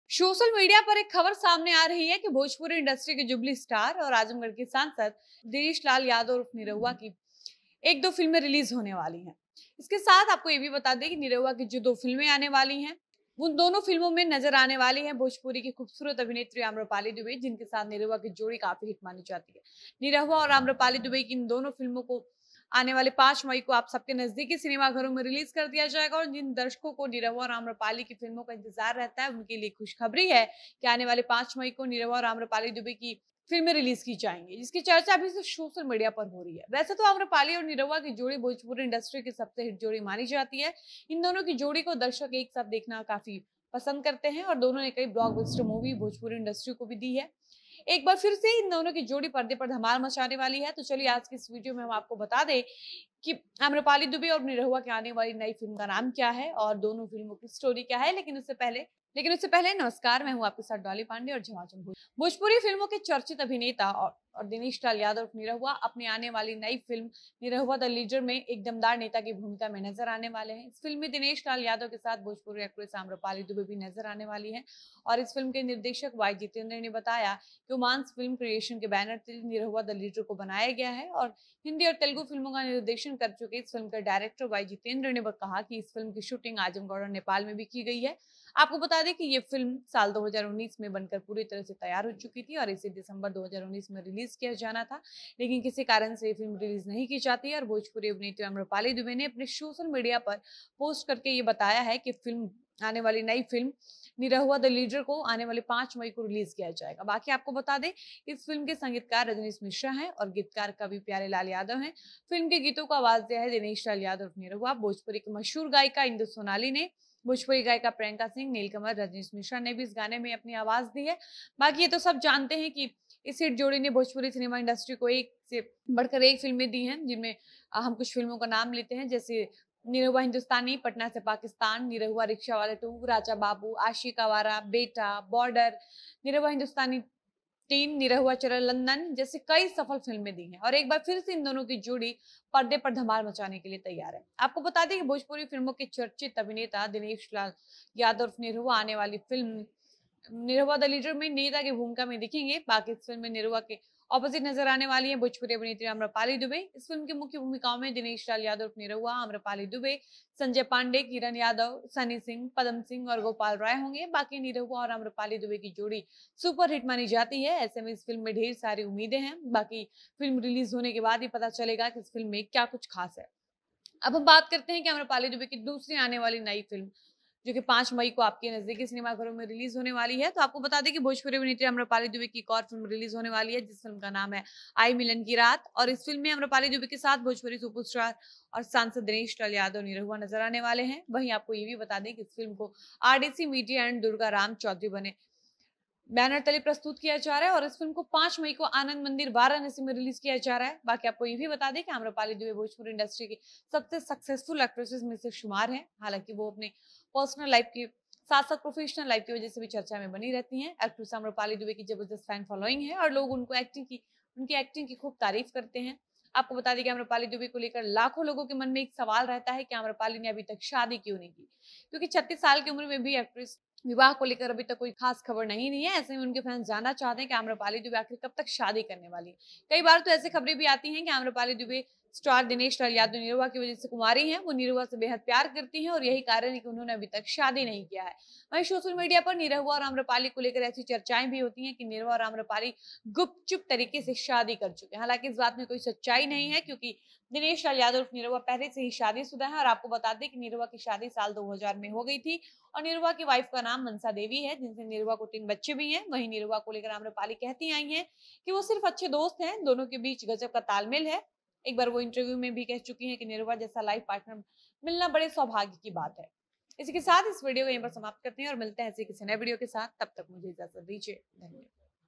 Aamrpali Dubey और Dinesh Lal की कौन सी नई फ़िल्म सिनेमा घरों में रिलीज होने वाली है, जान लीजिए!
आम्रपाली दुबे और निरहुआ की जोड़ी एक बार फिर से पर्दे पर धमाल मचाने वाली है! जानिए आने वाले 5 मई को इन दोनों की कौन सी फिल्म रिलीज होने वाली है?